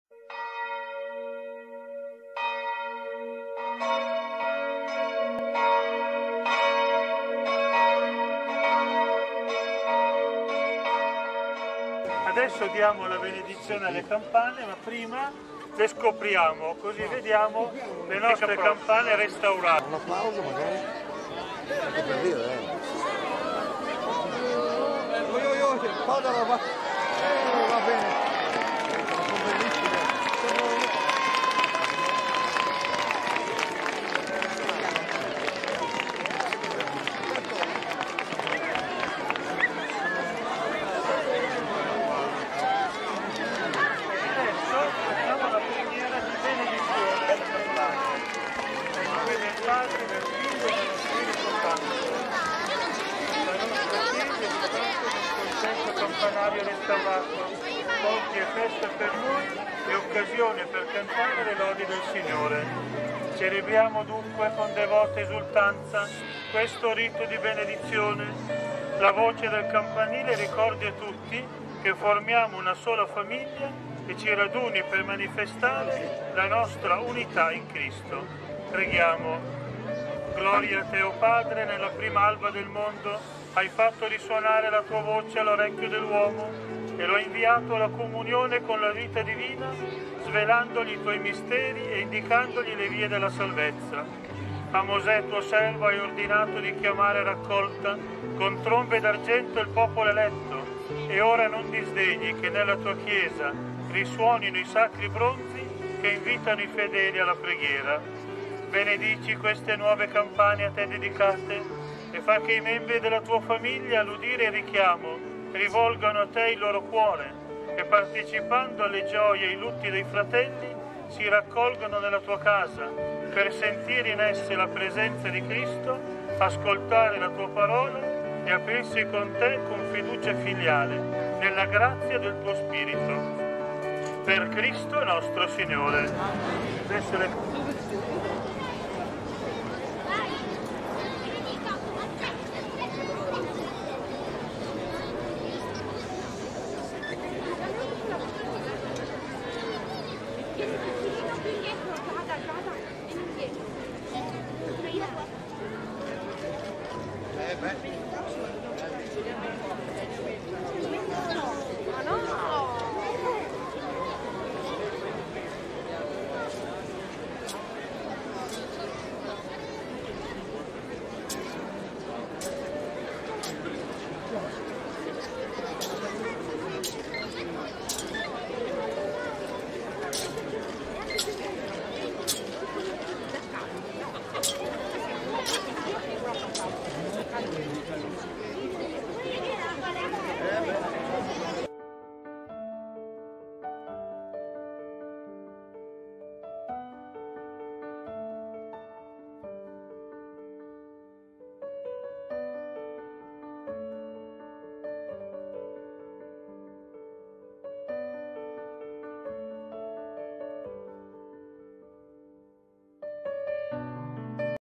Le campane di Cassolnovo (PV)- Benedizione
Video della benedizione delle campane restaurate della Chiesa Prepositurale di San Bartolomeo Apostolo, video di Santo Portento.

@tutti i diritti sono riservati - Parrocchia di Cassolnovo (PV)

Ringrazio l’autore del video e la parrocchia per le autorizzazioni.